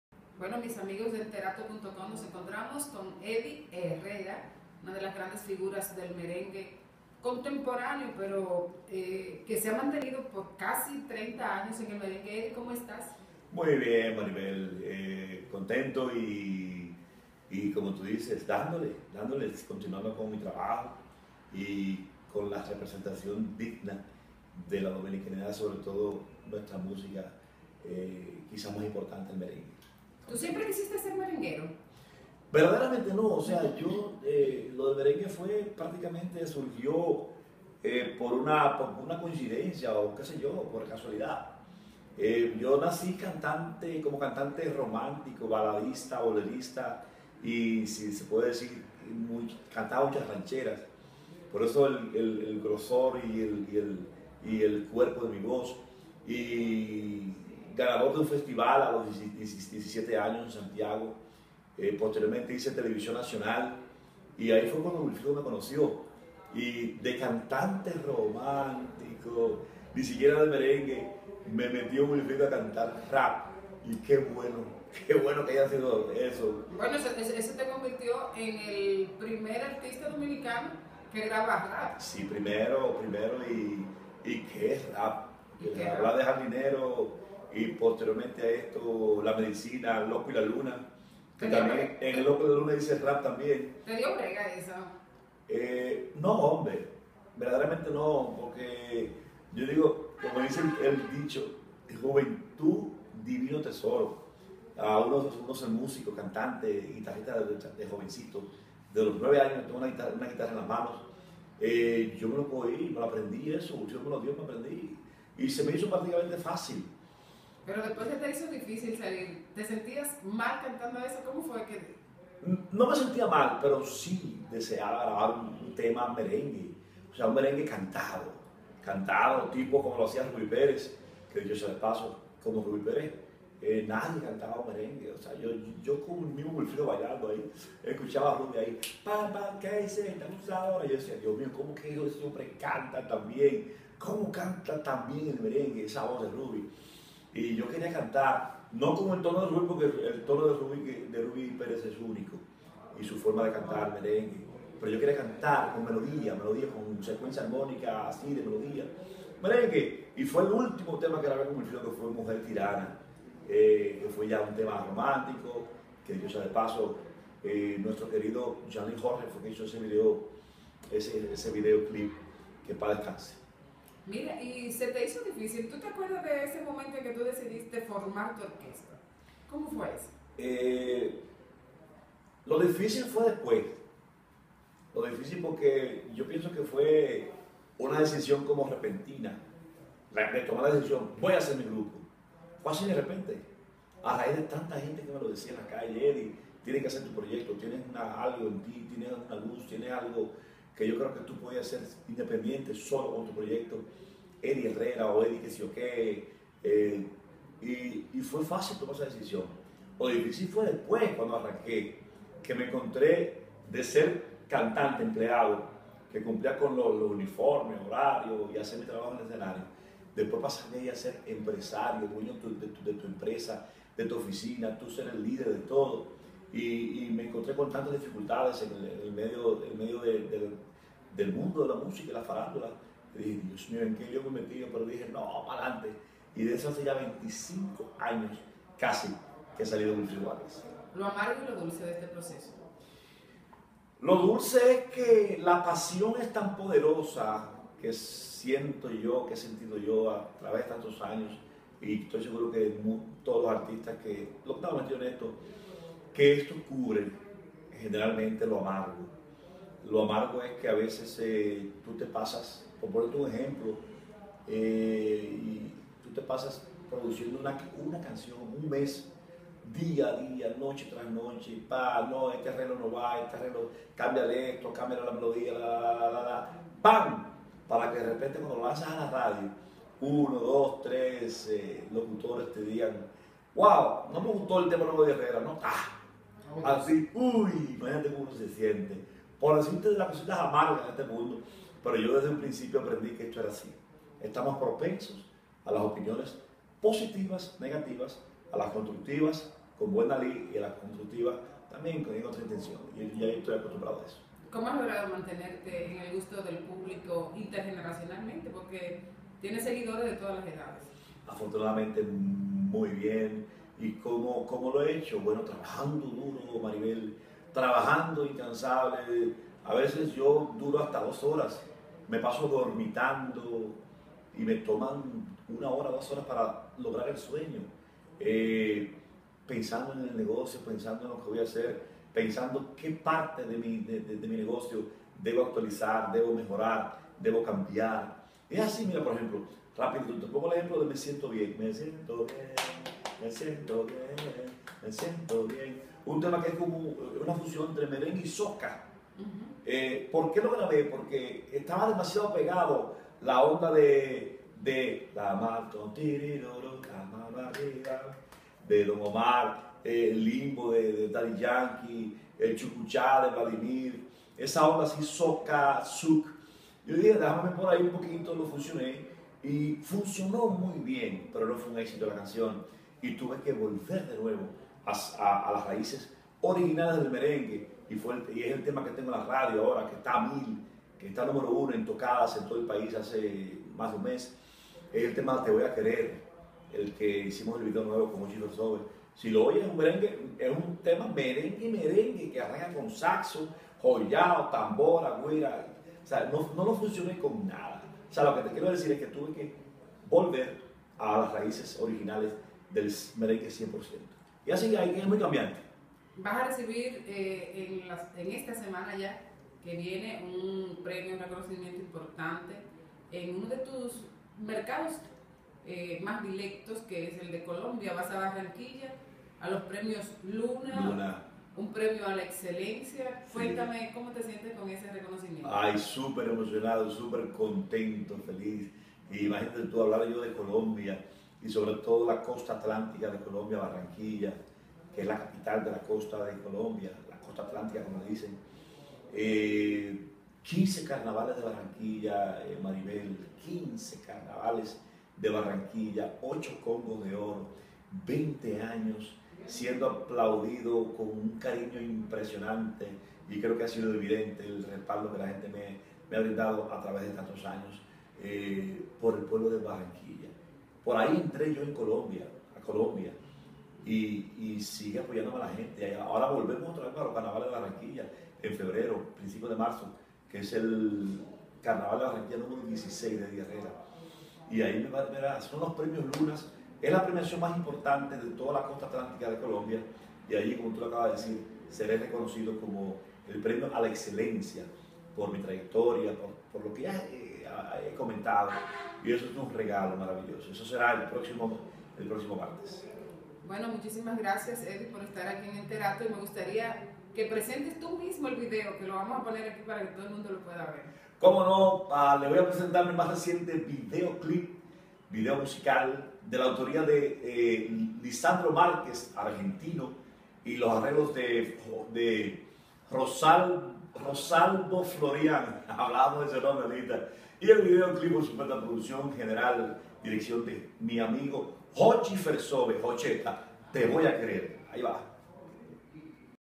Entrevista con Eddy Herrera Para Enterato
Detalles de su carrera y de un reconocimiento que recibirá de los Premios Luna, en Barranquilla...Colombia